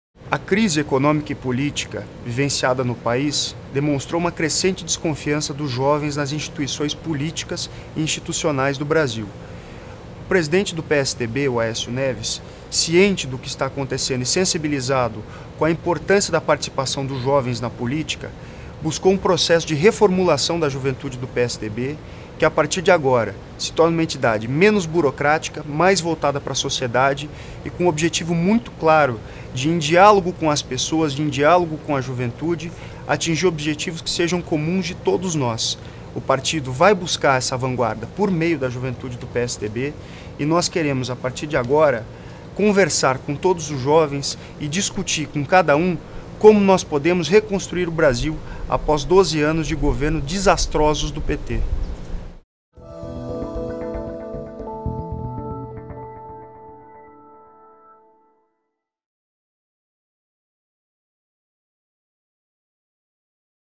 Henrique Vale: reconstrução após governos desastrosos do PT
A crise ampliou a desconfiança dos jovens em relação à política e às instituições, diz o presidente da Juventude do PSDB, Henrique Vale. Dinamizar sua atuação é o caminho dos jovens tucanos. “Queremos debater a reconstrução do Brasil após anos de governos desastrosos do PT”, diz ele.